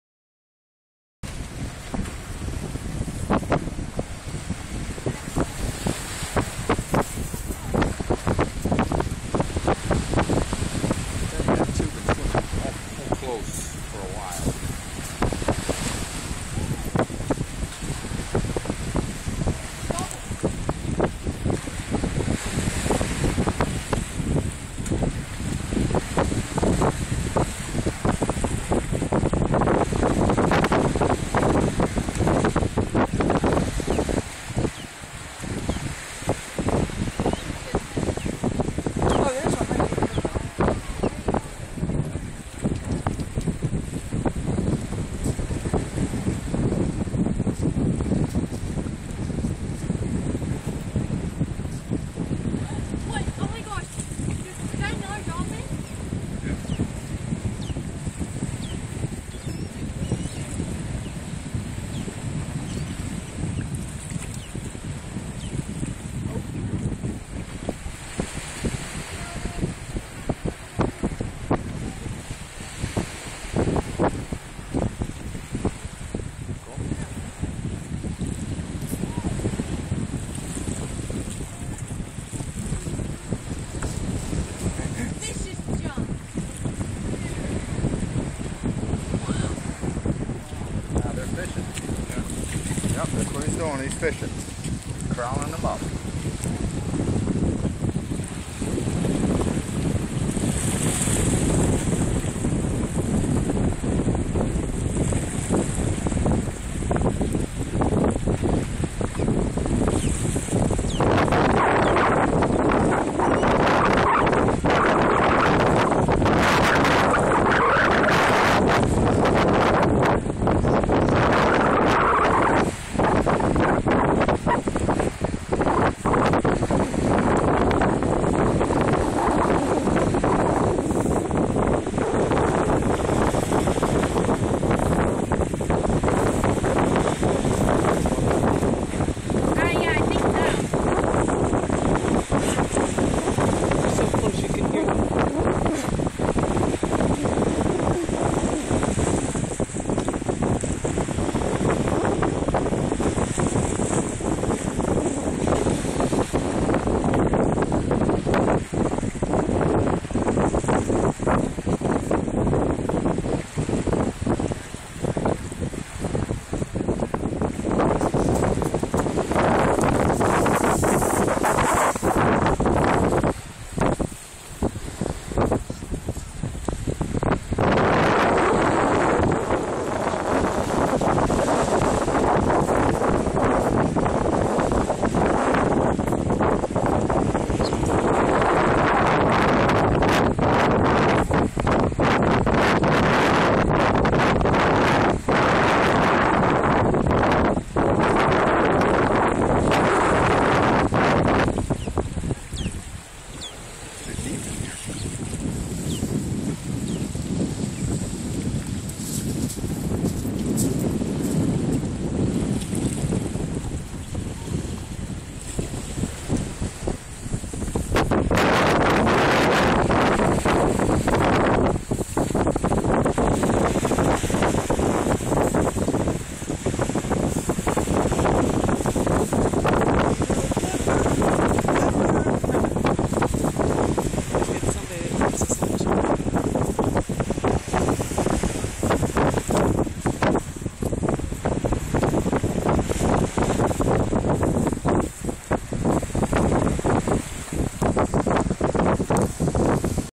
dolphins at sanibel 6-2012.3gp
we where a Sanibel  Island on Friday June 15, 2012 Great Sun, Water and the Dolphins were having fun.